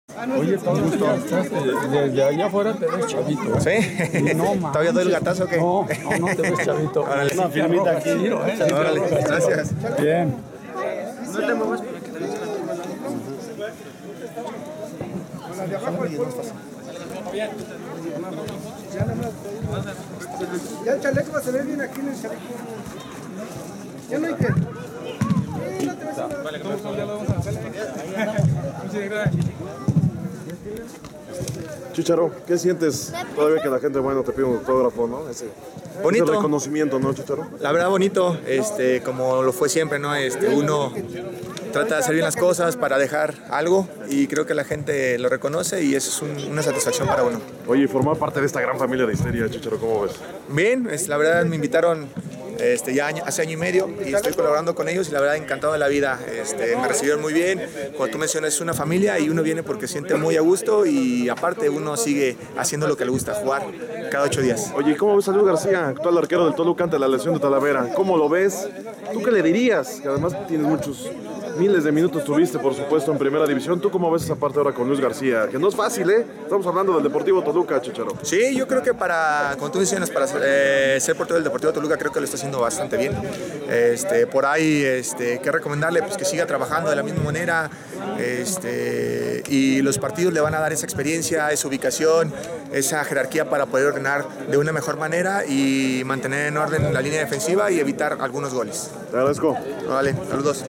CHICHARO LOZANO DE CD HISTERIA LE MANDA UN CONSEJO A LUIS GARCÍA ACTUAL ARQUERO DE LOS DIABLOS
CÉSAR EL CHICHARO LOZANO ARQUERO RETIRADO QUIEN MILITÓ EN EL CLUB DEPORTIVO TOLUCA HABLA DEL ACTUAL PORTERO DE LOS DIABLOS LUIS GARCÍA Y TAMBIÉN DE FORMAR PARTE DE LA INSTITUCIÓN HISTERIA